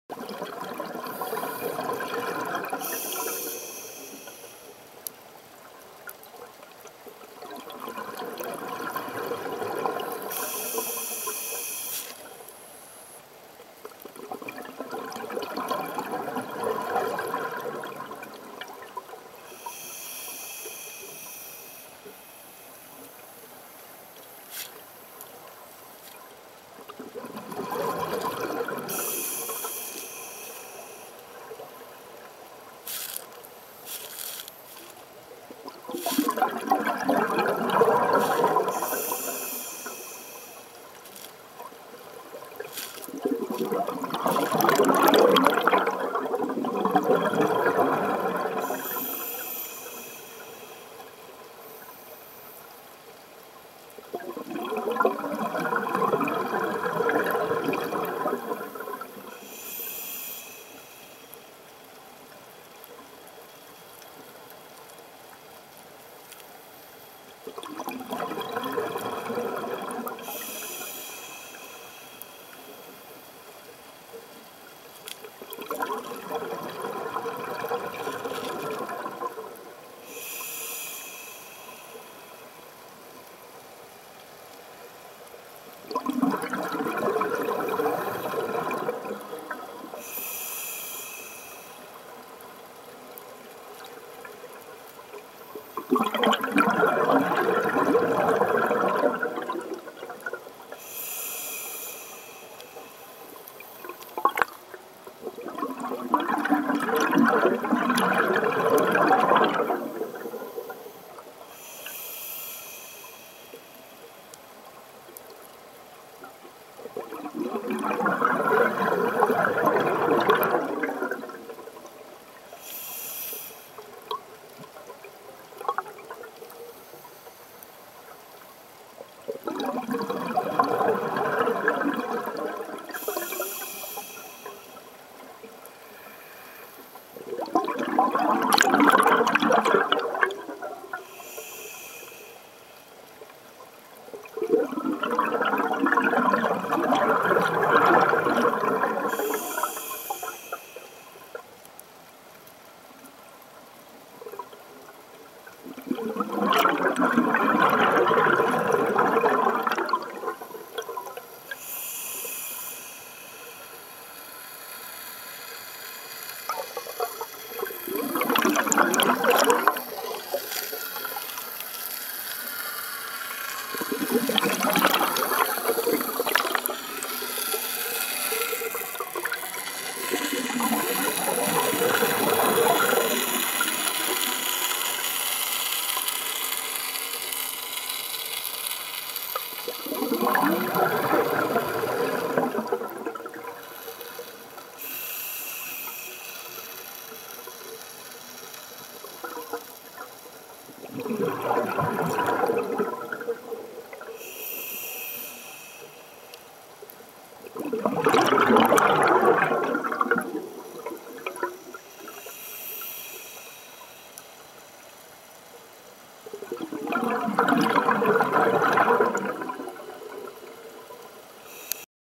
Surrounded by a school of Amberjack at Kas, Turkey, Part 1
That's Kas, full of surprises!!!